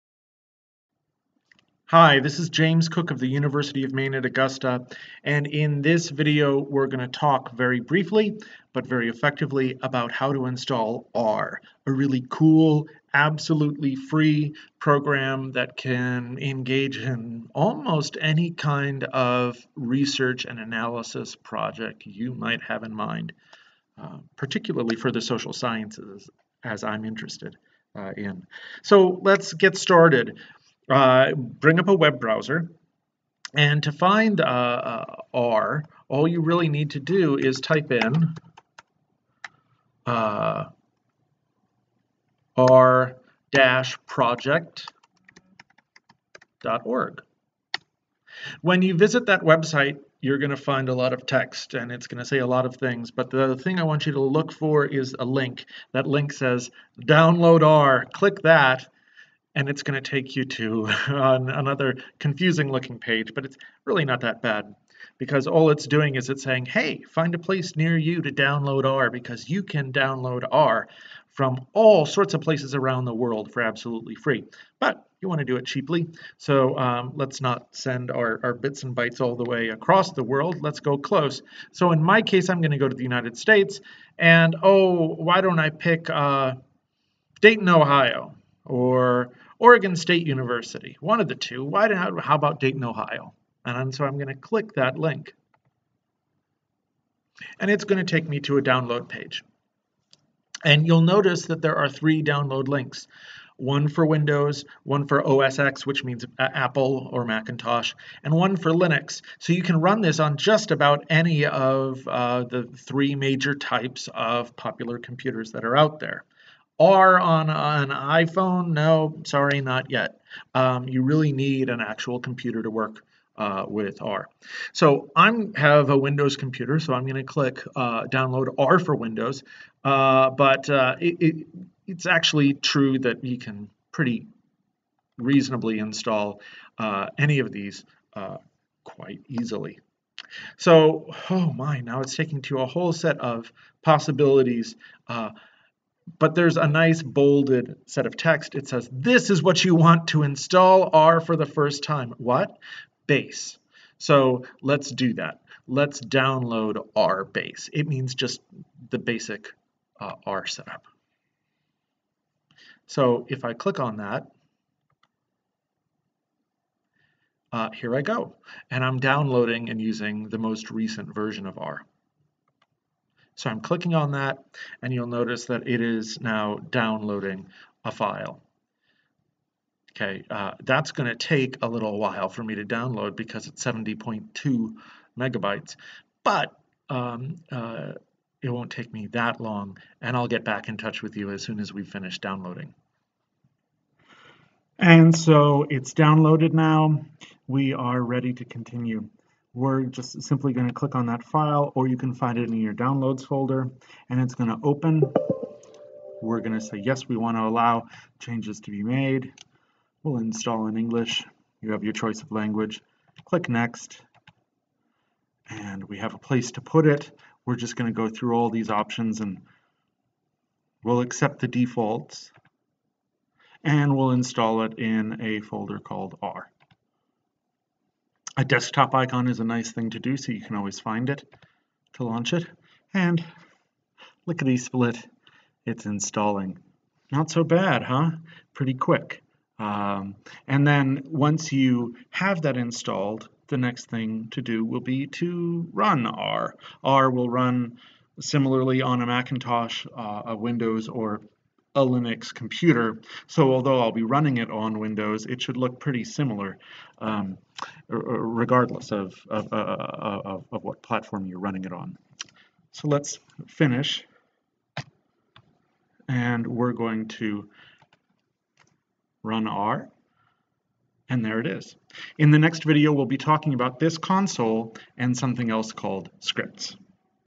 Installing R for Research
This video briefly describes how to install R, a piece of software that is free of charge, designed by statisticians and methodologists, and powerfully capable of various research tasks. To install R, you have to navigate through the potentially confusing text overload of the R Project , but once you've made it through the selection of a site and the appropriate "base" download, the rest of installation is a breeze.  This installation procedure shows the process for a Windows computer, but it's also possible to install R for Linux or for an Apple computer.